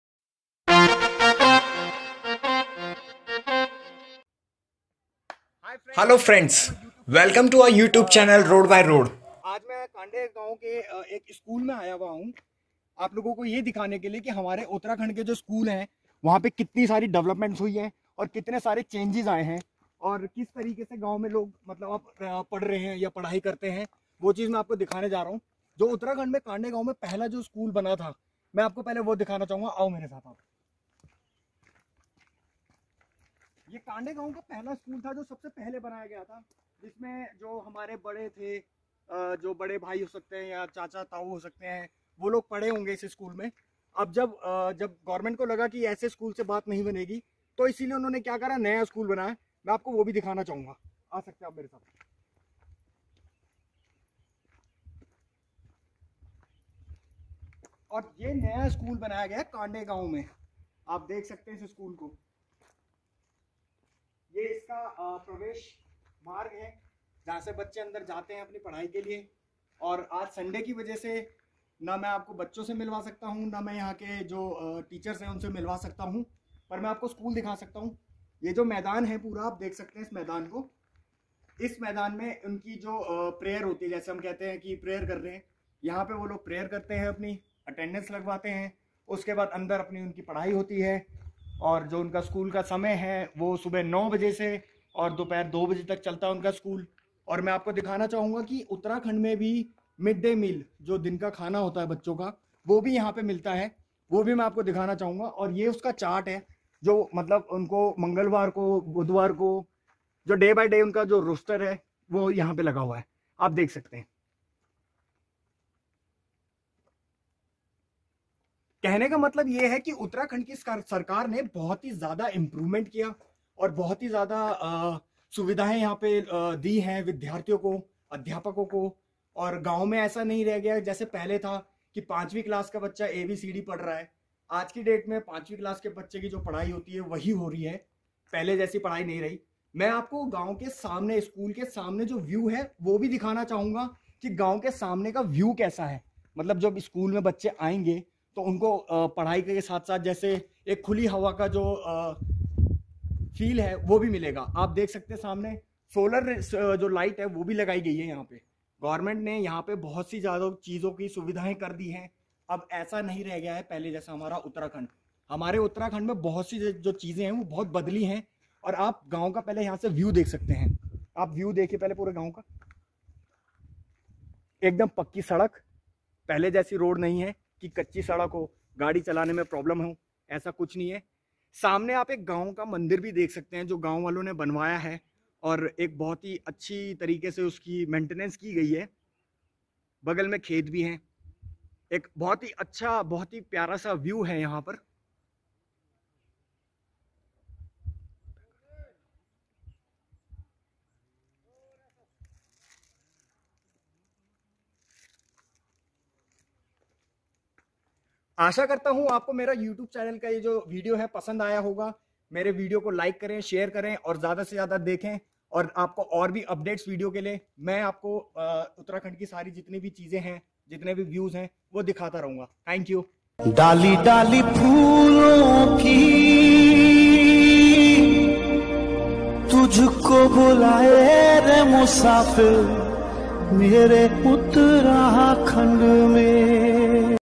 एक झलक कांडई गाँव के प्राथमिक विद्यालय की | A glimpse of primary school of Kandai village
दोस्तों यह छोटी सी वीडियो क्लिप है कांडई गाँव के प्राथमिक विद्यालय की जो उत्तराखंड में है ! इस वीडियो क्लिप में कांडई गाँव के विद्यालय की कुछ झलकियां है जिसे हम आप तक लाए है ! उम्मीद है यह वीडियो क्लिप आप को पसंद आए !